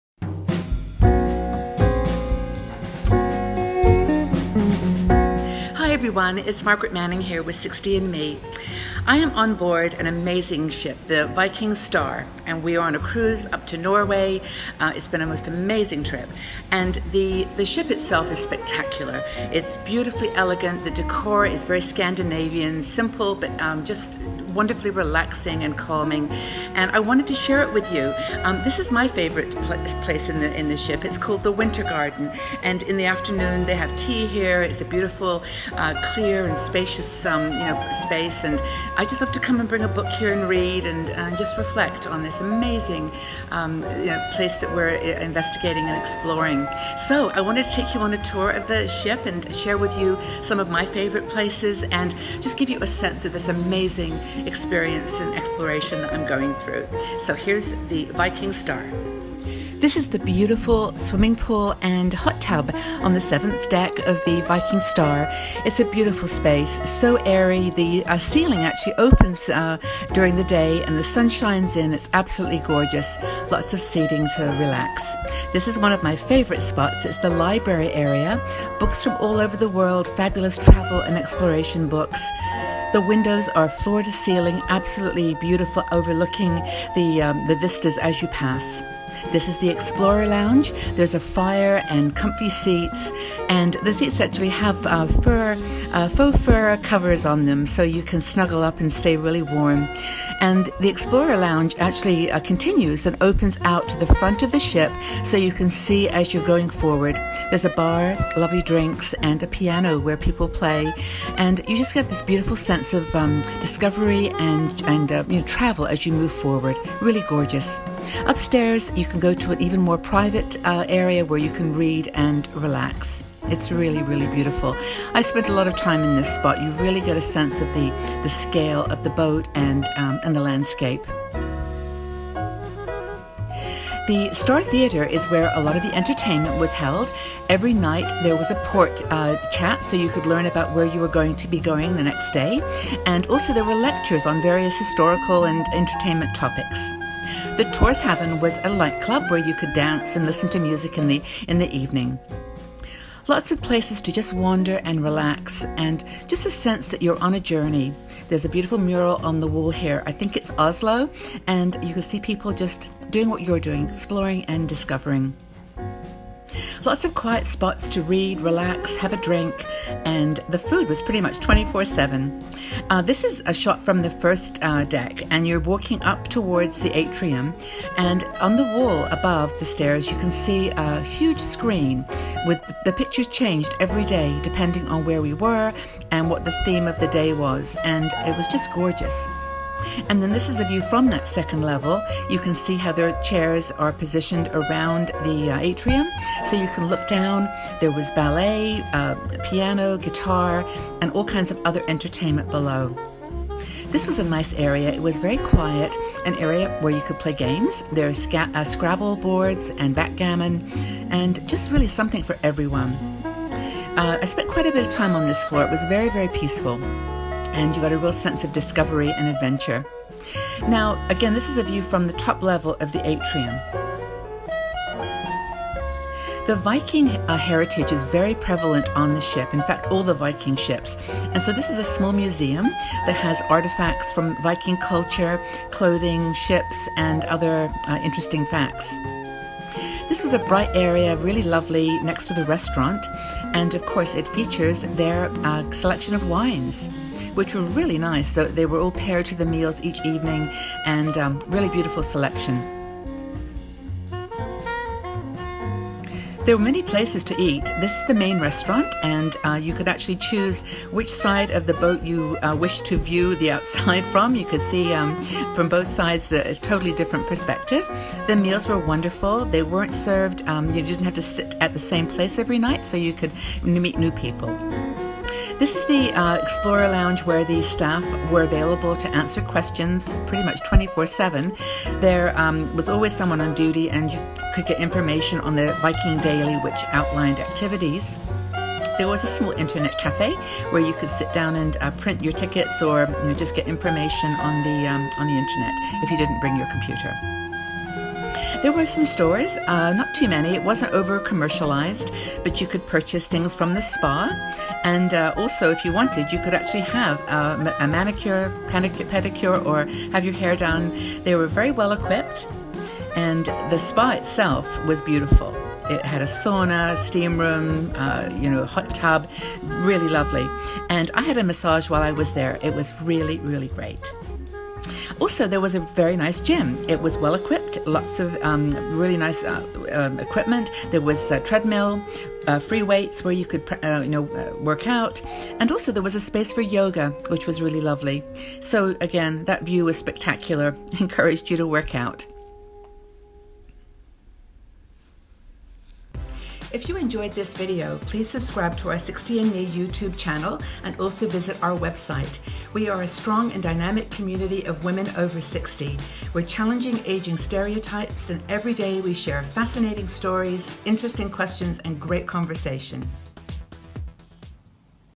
My Viking Star Cruise Review and Walkthrough - The Perfect Mix of Adventure and Luxury
What two words describe the Viking Star cruise ship? Adventure and luxury! The tour begins at 1:00. In this video, I will give a walkthrough of this amazing ship by Viking Cruises and share a few of my favorite places.

I had so much won on the Viking Star on this trip. Throughout my journey, I felt like I was an explorer from centuries past. During the day, we would visit exotic places, filled with mysteries and wild beauty. The, at night, we would retire to one of the most luxurious ships on the planet.

If you are looking for ideas for your next cruise, I hope that you will give the Viking Star by Viking Cruises a try. You won't regret it!

What did you think of my Viking Star walkthrough? Do you also like to cruise? What is the best cruise that you have ever taken? What questions do you have for me about my trip? Please join the conversation!

You can find out more about Viking Ocean Cruises on their